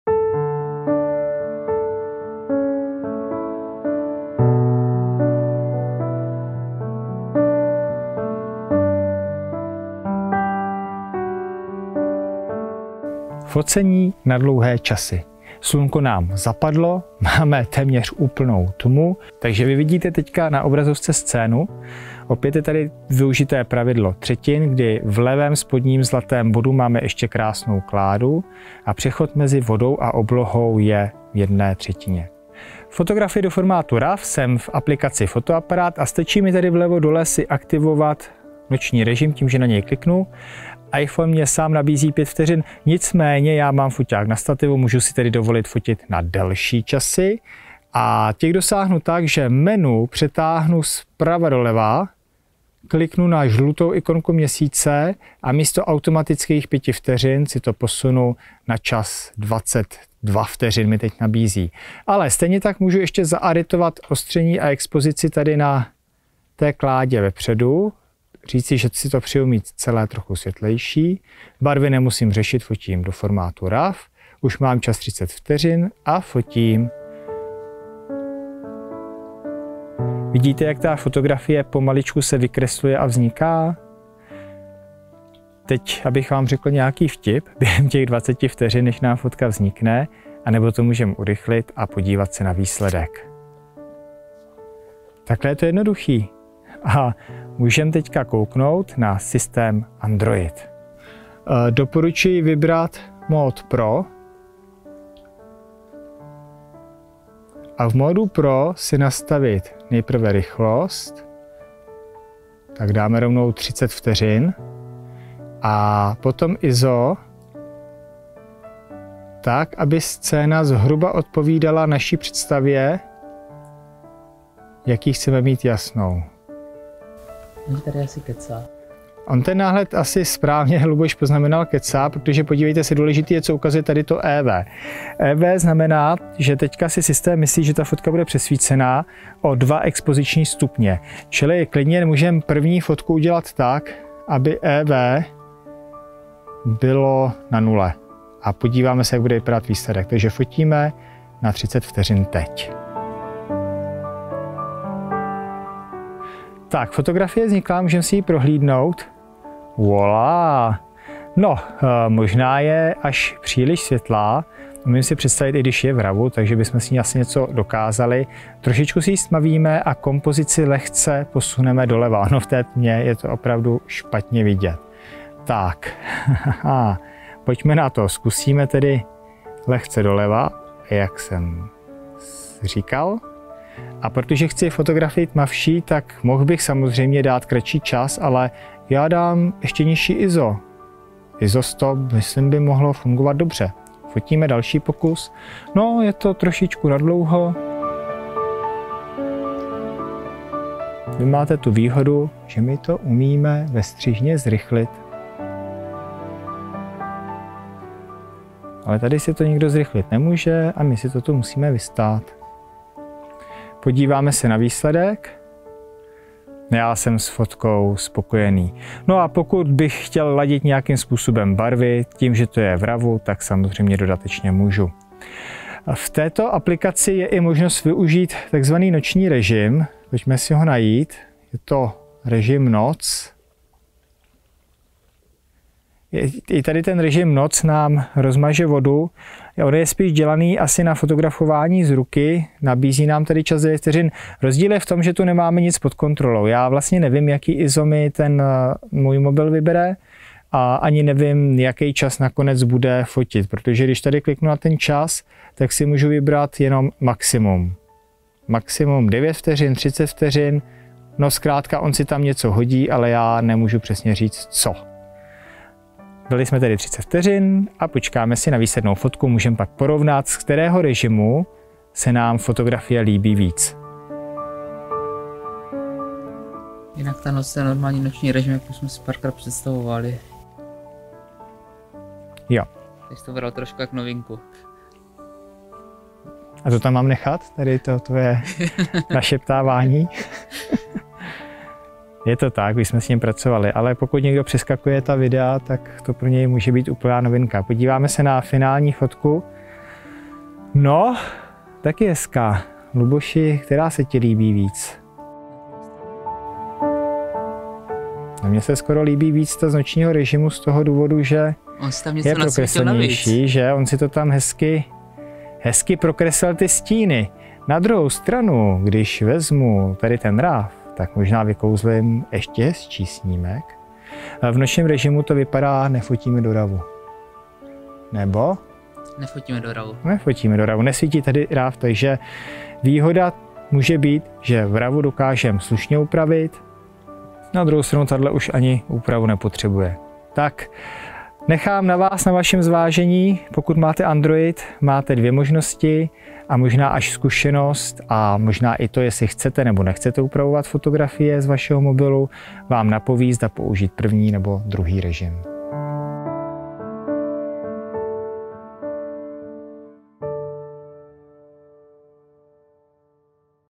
Jak fotit krajinu mobilem: 5. díl – Moře (záliv ve tmě – fotíme mobilem na opravdu dlouhé časy)
V tomto dílu kurzu „Jak fotit krajinu mobilem“ se zaměříme na techniku focení na opravdu dlouhé časy. Tato metoda je ideální pro zachycení pohybu vody nebo oblaků, které vytvářejí dynamický a dramatický efekt. Připravte si svůj mobil, stativ a jdeme na to! Tento díl je součástí celého kurzu, který najdete na webových stránkách na tomto odkazu

Nejprve se podíváme na to, jak lze využít tuto techniku s iPhonem. Prozkoumáme nastavení fotoaparátu, které nám umožní fotografovat na dlouhé časy, a ukážeme si, jak dosáhnout nejlepších výsledků.

Následně se zaměříme na systémy Android, jako je Samsung nebo Xiaomi. I zde si vysvětlíme, jak nastavit fotoaparát pro dlouhé expozice a jakým způsobem lze dosáhnout nejlepších snímků. Takže, je libo fotit třeba na 30 vteřin? Proč ne! Připojte se k nám a naučte se, jak vytvořit úchvatné fotografie pomocí svého mobilu.

Děkujeme, že nás sledujete, a těšíme se na vás u dalších dílů! Prohlédněte si všechny díly na našem webu na tomto odkazu: